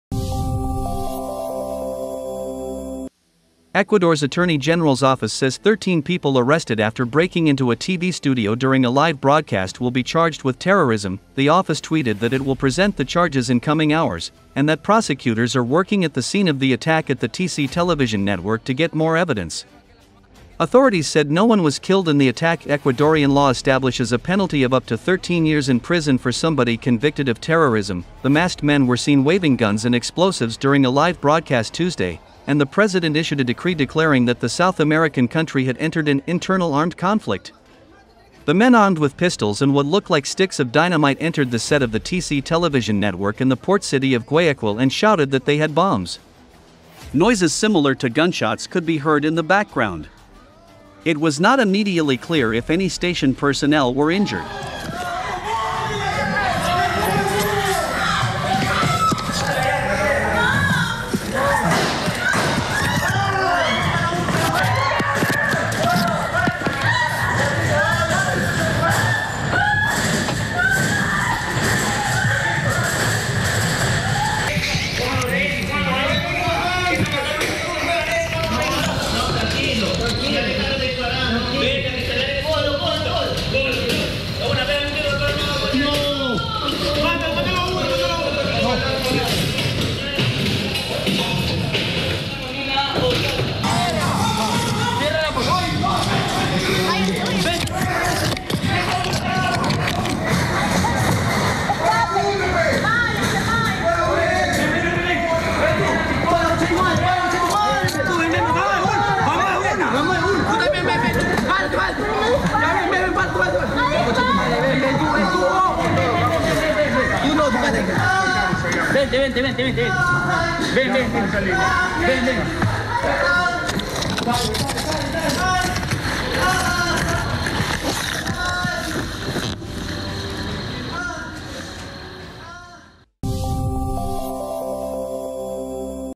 Stark images of the moment an Ecuador TV station is attacked by masked gunmen
Ecuador's attorney general's office says 13 people arrested after breaking into a TV studio during a live broadcast will be charged with terrorism.

The office tweeted that it will present the charges in coming hours, and that prosecutors are working at the scene of the attack at the TC Television network to get more evidence. Authorities said no one was killed in the attack.

Ecuadorian law establishes a penalty of up to 13 years in prison for somebody convicted of terrorism.

The masked men were seen waving guns and explosives during a live broadcast Tuesday, and the president issued a decree declaring that the South American country had entered an “internal armed conflict.”

The men armed with pistols and what looked like sticks of dynamite entered the set of the TC Television network in the port city of Guayaquil and shouted that they had bombs. Noises similar to gunshots could be heard in the background. It was not immediately clear if any station personnel were injured.